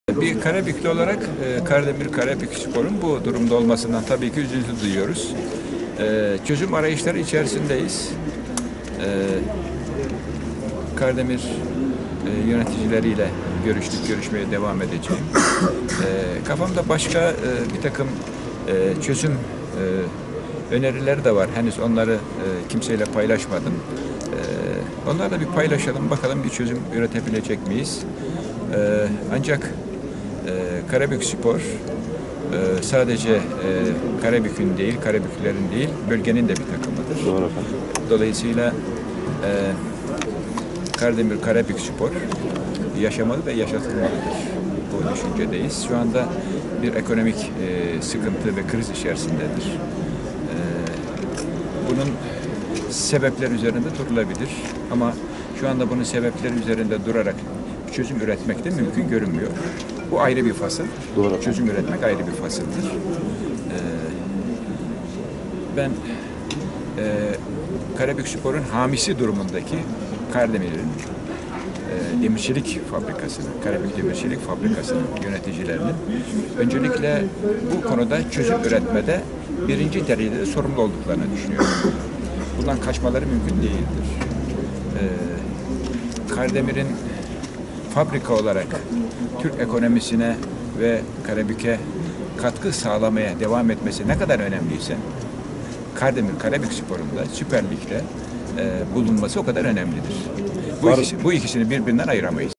MEHMET ALİ ŞAHİN KARDEMİR KARABÜKSPOR
KARABÜK MİLLETVEKİLİ M.ALİ ŞAHİN KARDEMİR KARABÜKSPOR SADECE KARABÜK'ÜN DEĞİL BÖLGESİNİN TAKIMIDIR. DEDİ .